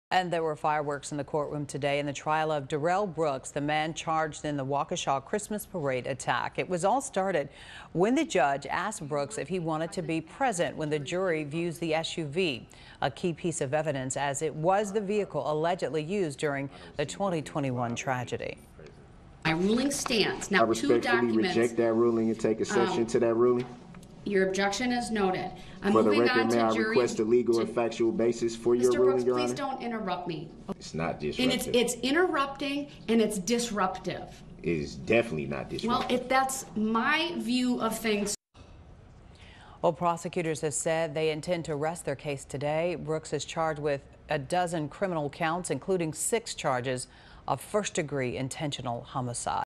Waukesha parade suspect's heated exchange with judge
There were fireworks in the courtroom Wednesday in the trial of Darrell Brooks, the man charged in the Waukesha Christmas parade attack.